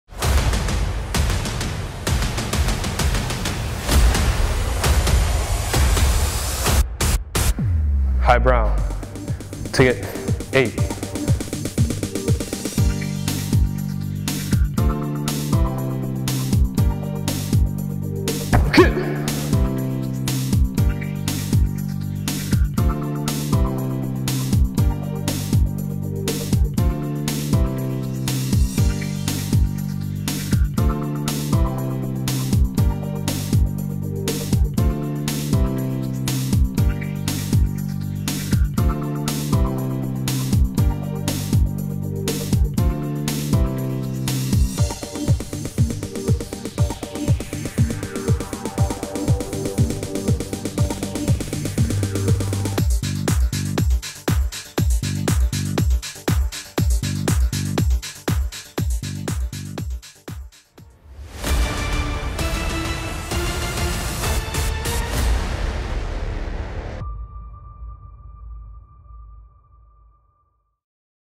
High Brown Belt Taegeuk 8 - Taekwondo Kata 🥋
In this video you will learn the High Brown Belt Taegeuk 8 Kata of Taekwondo.

👉 If you’d like to learn Taekwondo, Hapkido or self defense in our Bronx school, visit us